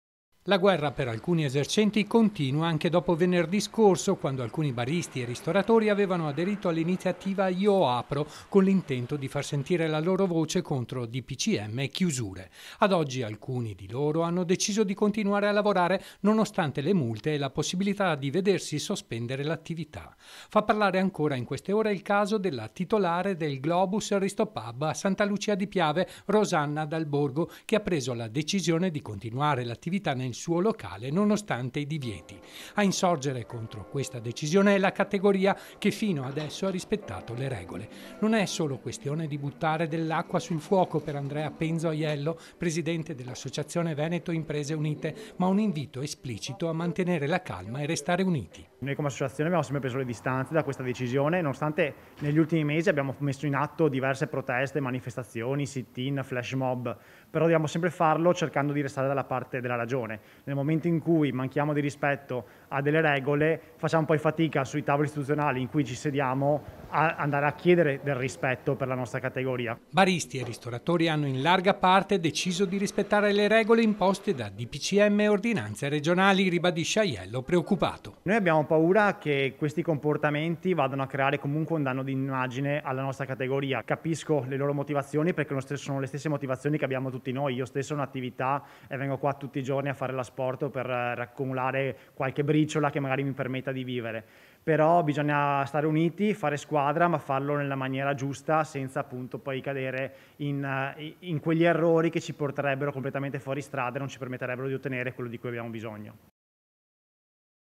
LA CATEGORIA CONTRO I RIBELLI: «SOLO UNITI POSSIAMO FARCELA» | 19/01/2021
A3 NEWS Treviso 19/01/2021 - TREVISO - Dopo la decisione di alcuni esercenti di continuare a tenere aperto il loro locale nonostante i divieti insorge la categoria, che con tutti gli associati hanno fino adesso rispettato le regole.    || La guerra per alcuni esercenti continua anche dopo Venerdì scorso, quando alcuni baristi e ristoratori avevano aderito all’iniziativa Io Apro, con l'intento di far sentire la loro voce contro DPCM e chiusure. Ad oggi, alcuni di loro, hanno deciso di continuare a lavorare, nonostante le multe e la possibilità di vedersi sospendere l'attività. Fa parlare ancora in queste ore il caso della titolare del Globus Ristopub a Santa Lucia di Piave, Rosanna dal Borgo, che ha preso la decisione di continuare l'attività nel suo locale nonostante i divieti. A insorgere contro questa decisione è la categoria che fino adesso ha rispettato le regole. Non è solo questione di buttare dell'acqua sul fuoco per Andrea Penzo Aiello Presidente dell'associazione Veneto Imprese Unite, ma un invito esplicito a mantenere la calma e restare uniti.Baristi e ristoratori hanno in larga parte deciso di rispettare le regole imposte da DPCM e ordinanze regionali, ribadisce Aiello, preoccupato. - Intervistati Andrea Penzo Aiello (Pres. Ass. "Veneto Imprese Unite") (Servizio di Nicola Marcato) - Segui Antenna Tre anche sul digitale terrestre! Visita il sito